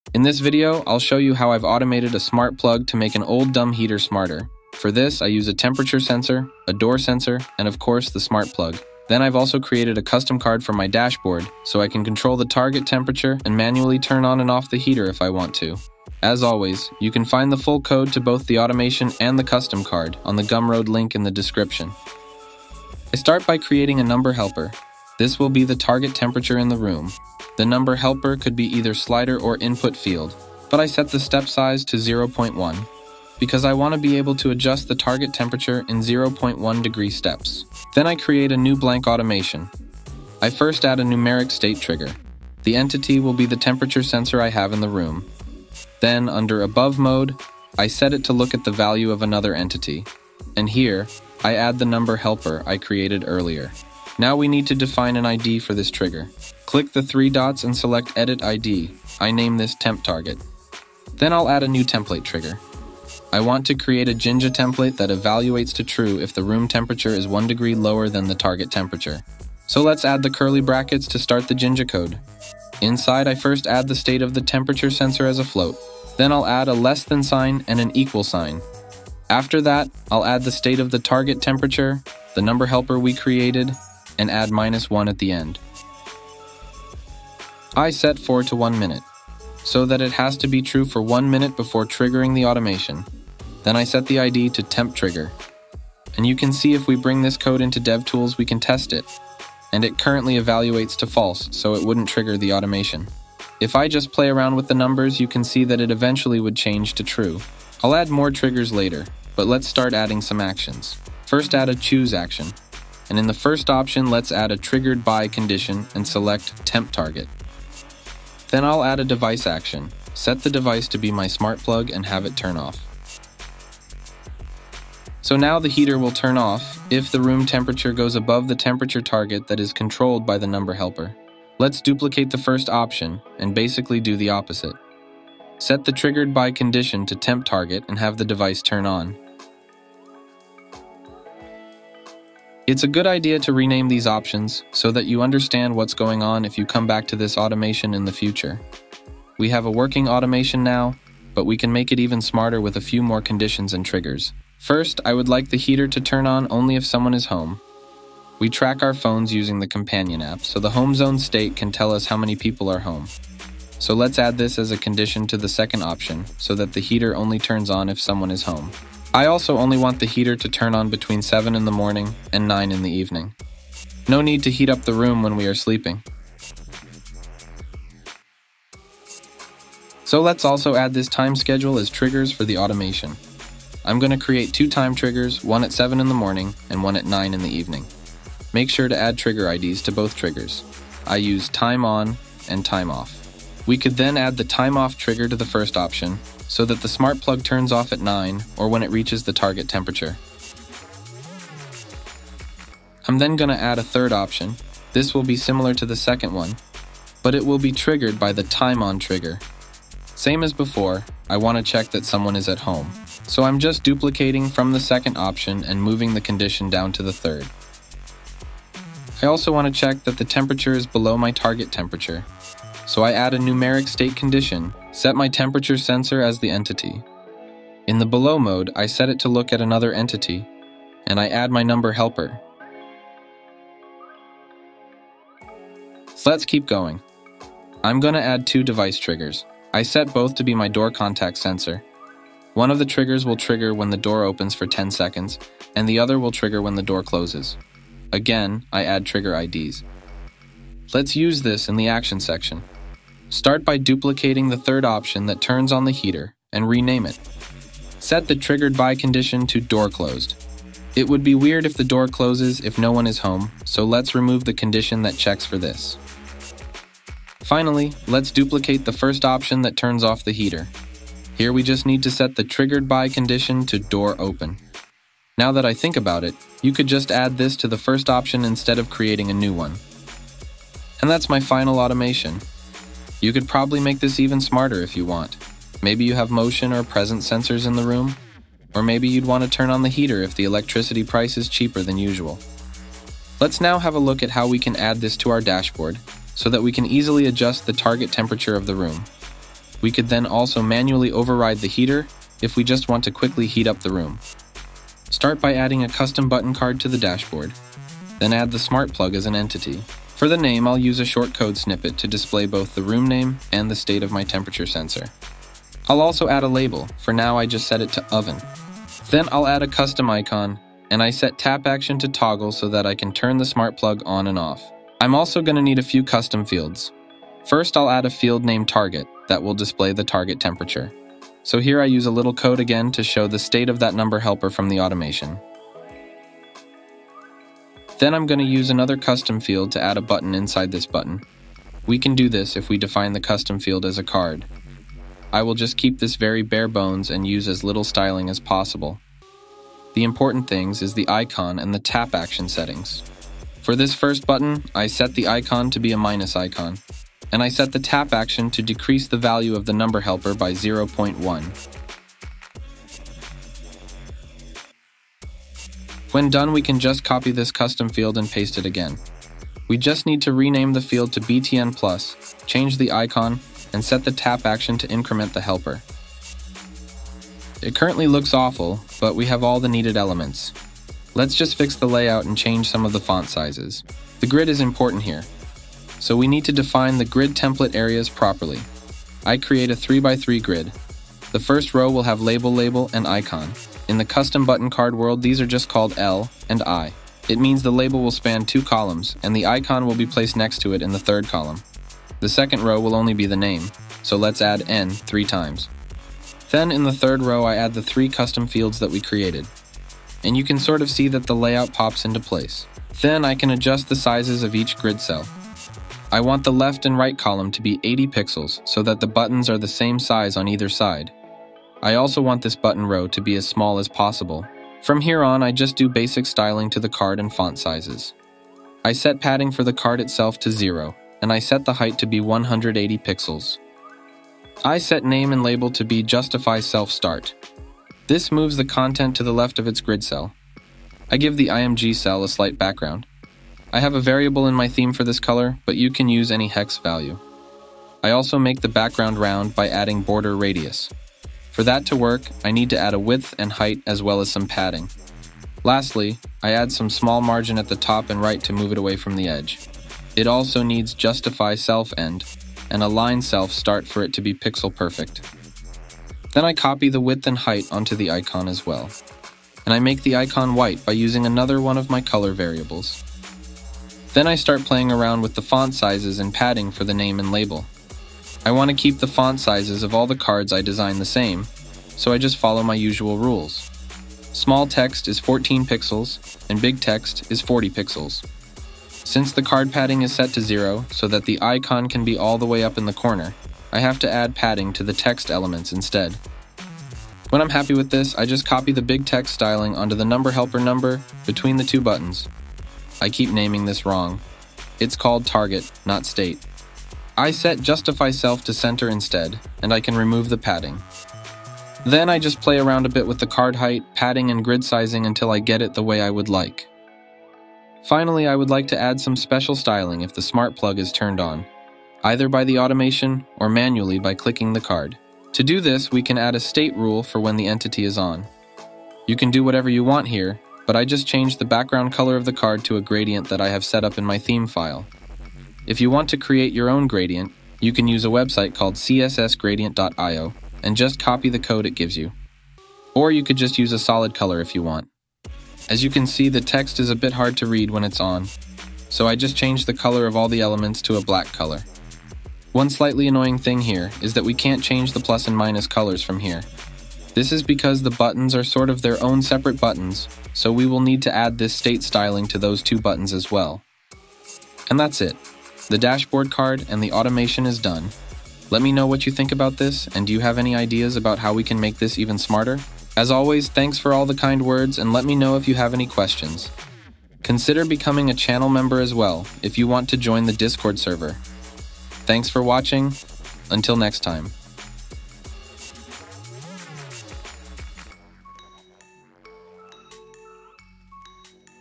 Make a dumb heater smart(er) with Home Assistant
In this video I'll show you how I've made a dumb heater smarter by using a smart plug, a temperature sensor and a contact sensor. Then I'll show how I control this "fake" thermostat from my dashboard.

A word of caution. Don't just plug anything into a smart plug. Make sure that the plug is spec'd for the load that you are plugging into it. The plug I use is rated for 16A and I have a wall mounted panel heater that pulls max 1000W. Make sure to not cover the plug and check regularly for any signs of heat or discoloration.